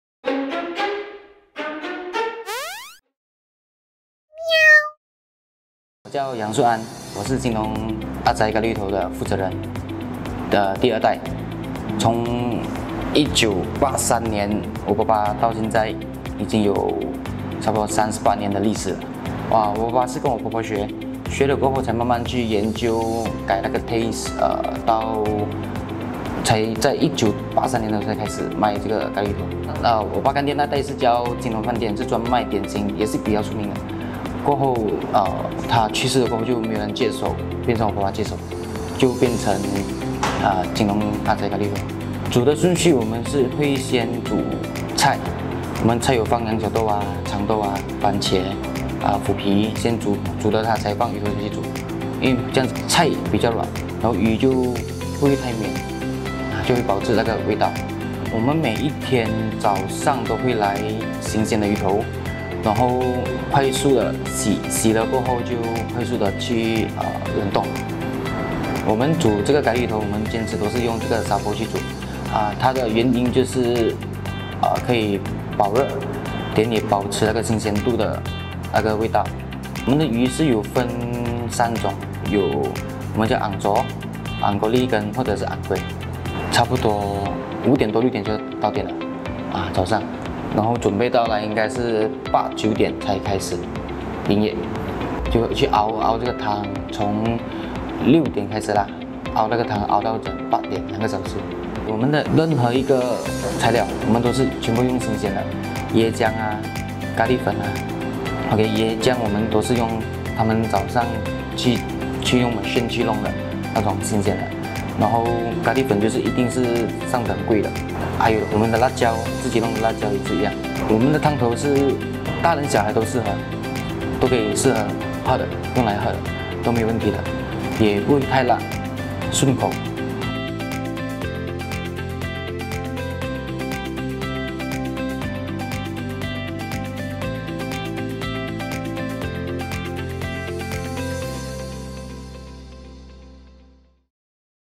Meow Meow X Johor Kam Long Ah Zai : Famous with only one item on its menu for over 25 years
Most tourists to Johor Bahru would have heard of Kam Long Ah Zai’s curry fish head. The restaurant was founded in 1983, especially popular with Singaporean foodies. The restaurant is always a full house and there are long queues everywhere. But at Meow Meow App, you can save yourself a long line and enjoy it with just one click.